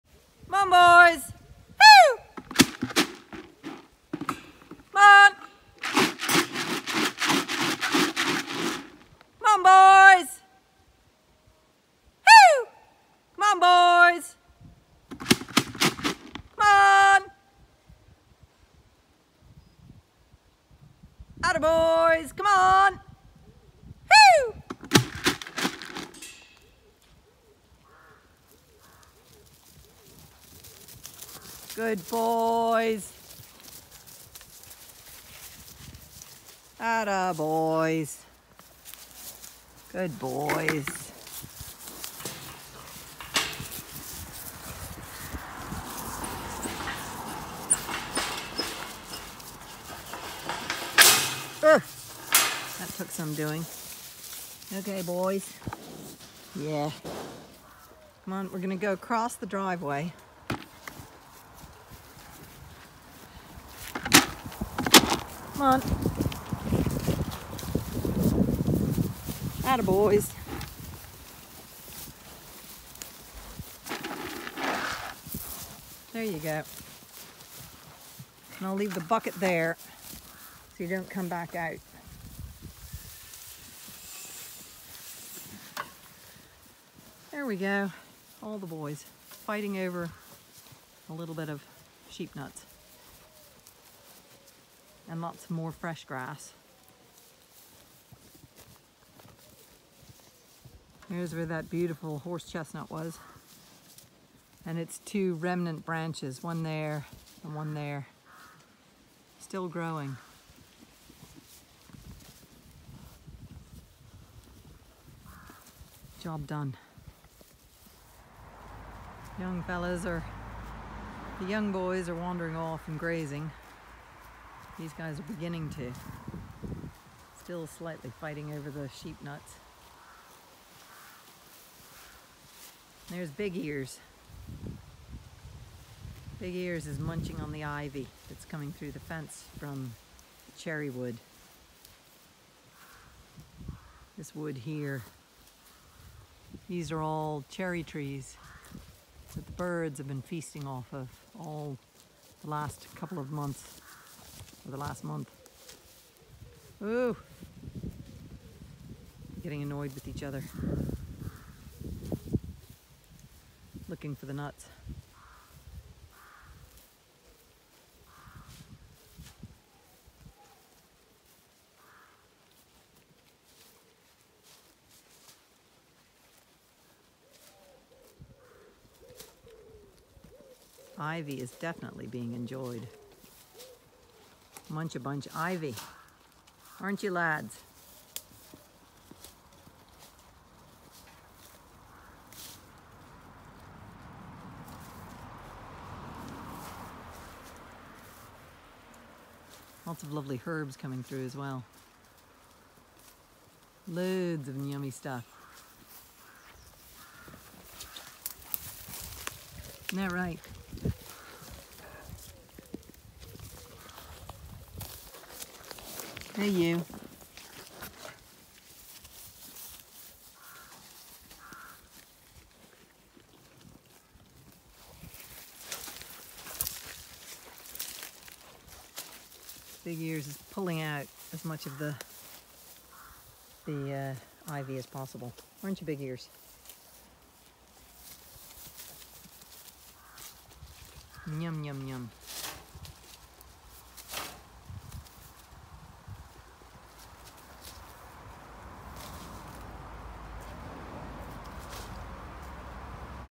Call rams and they come galloping across field to move to fresh pasture
Magic bucket does it's thing.